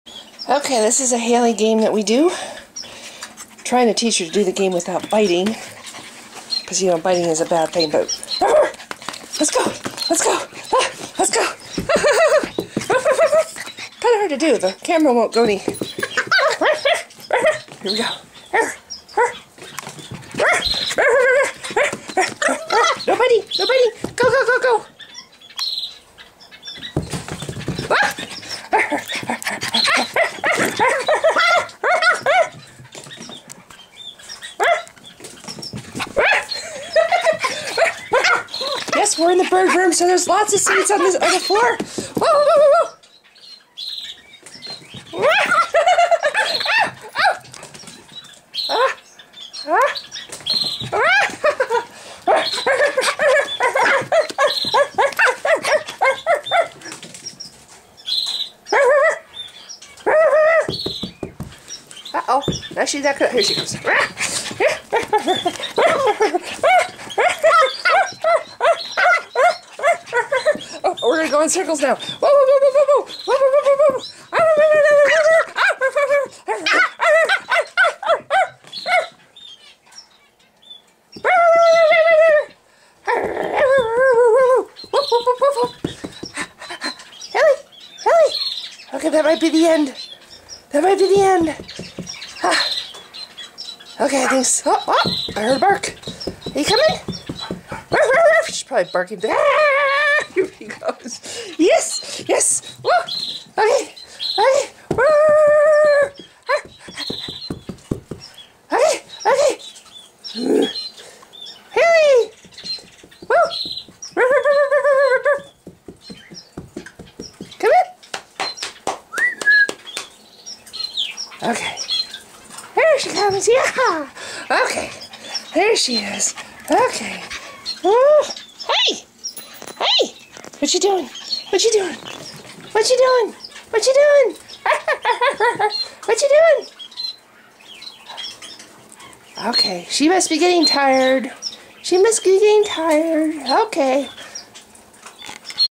One of Haley's Games 02/24/12
In my bird-room and Saturday before cleaning anything so yes seeds on the floor!  This game and running around mom in a circle are two of Haley's favorite games she likes to play in the morning.  When she was little I indulged her and let her bite at my hand but she's learning to play the game rough without biting.  She still sometimes bites but we stop the game when she does and she never bites hard when she does bite.   Happily she never shows biting behavior at other times so I don't think indulging her as a little puppy has made her mean or vicious.  She just loves to play and feel like she's a BIG dog!